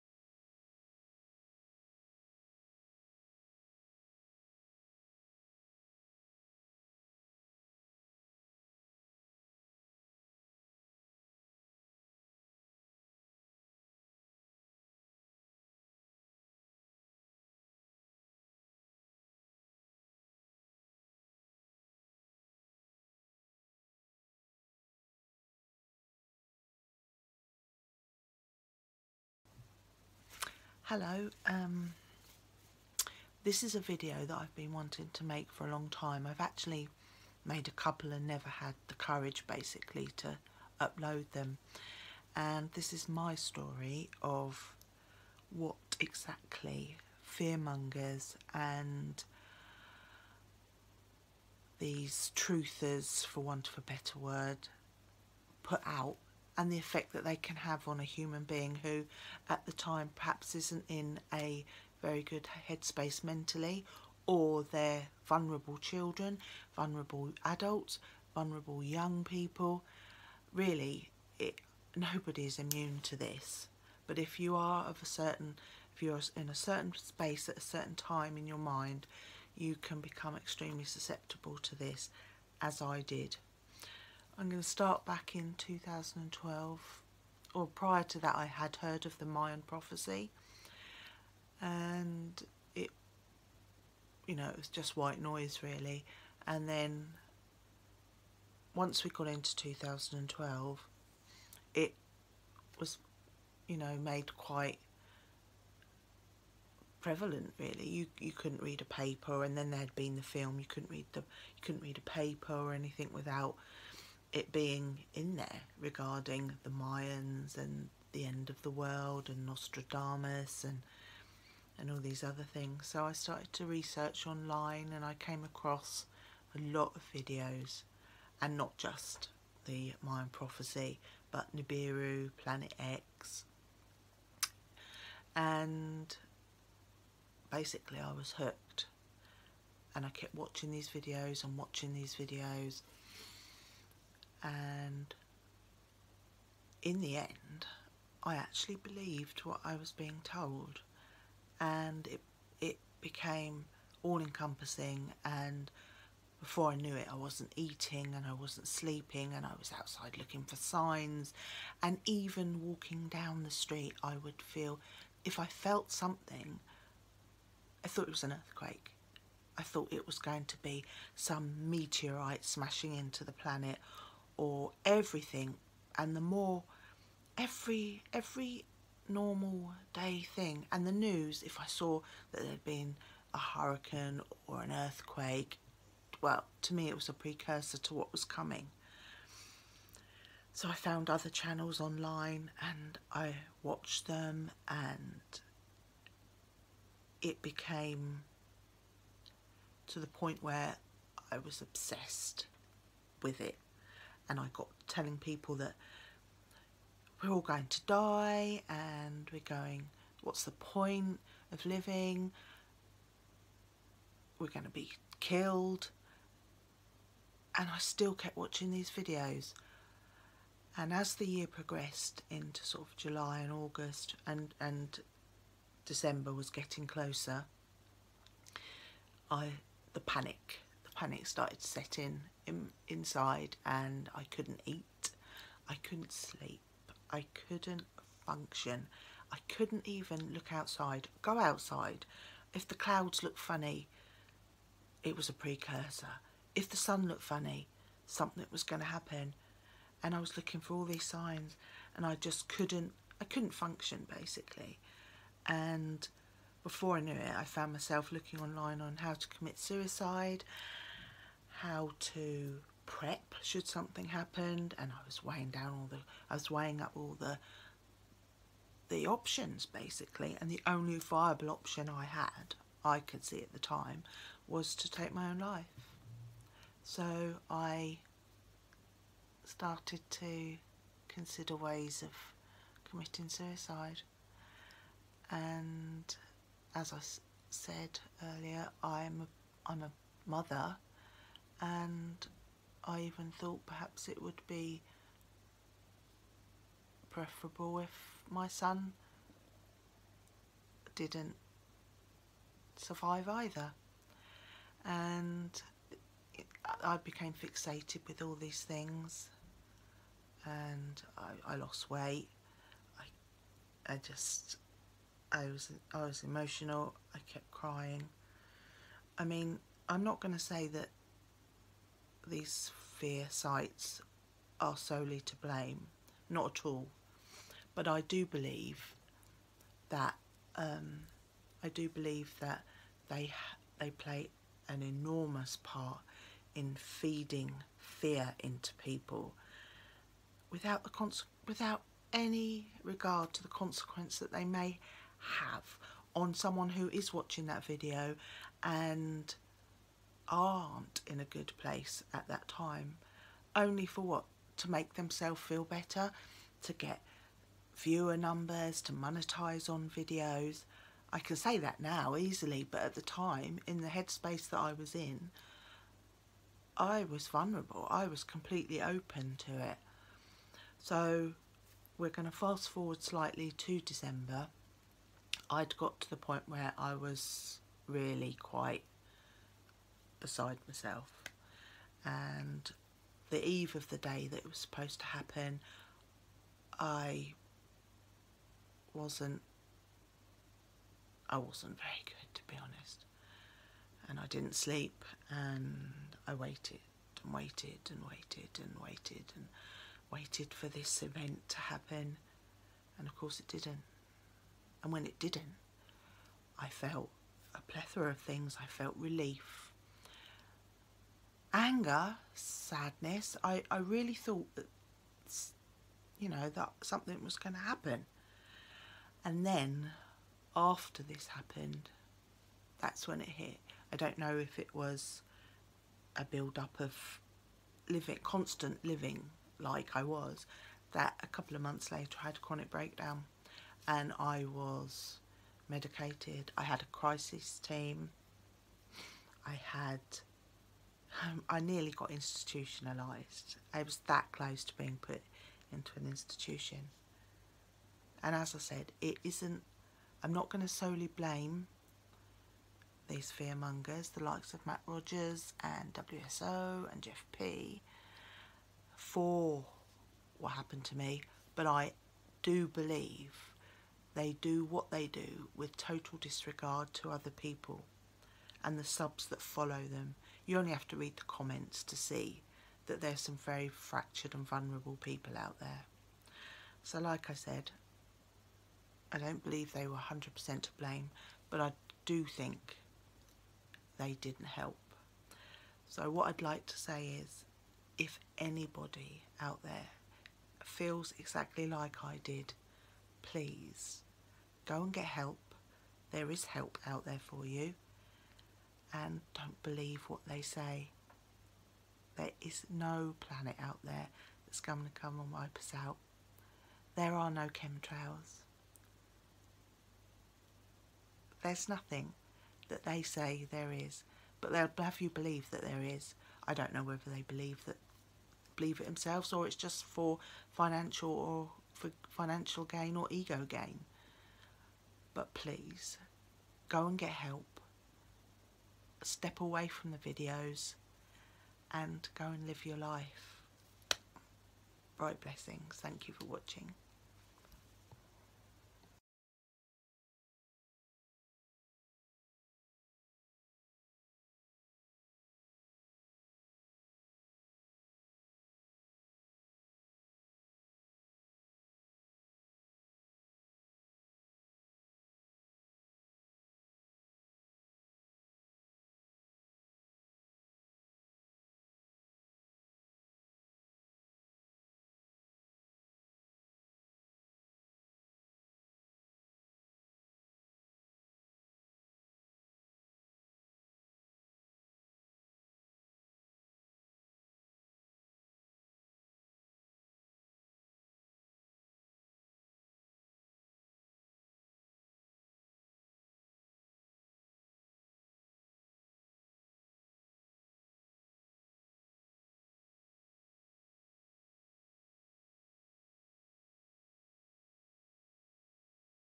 A Survivors Testimony FEAR PORN affects lives of real people. Mirrored for Thought and Memory.
A survivors testimony.  A true account of being vulnerable in the wrong place at the wrong time.  Fear Porn damages lives no matter how you like to dress it up, ignore, and blame the victims.